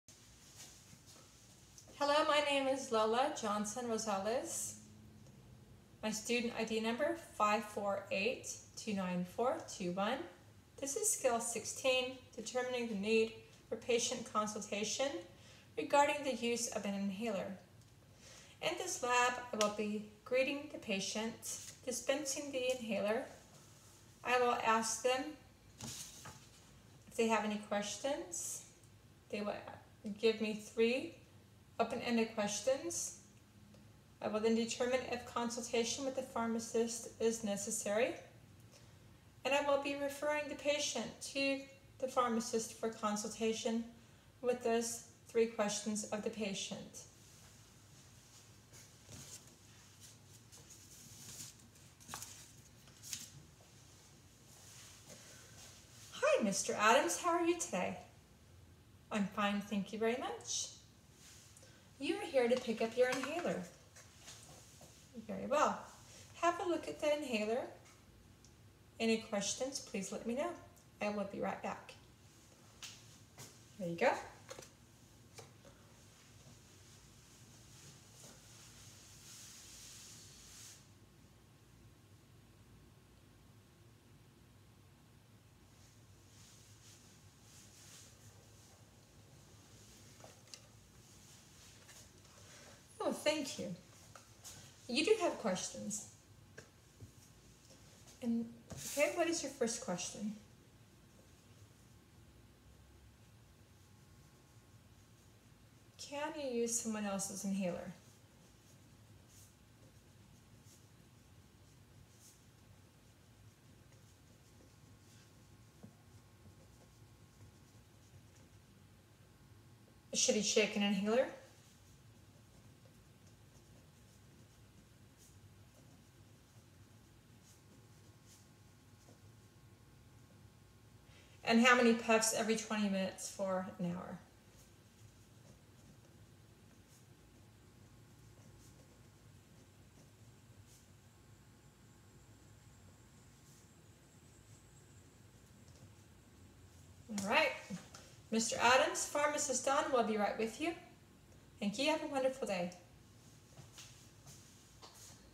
Skill 16:Determining the Need for Patient Consultation Regarding the Use of an Inhaler.54829421.LJR
Learn about how to greet and discuss a prescription with a patient regarding a prescription for an inhaler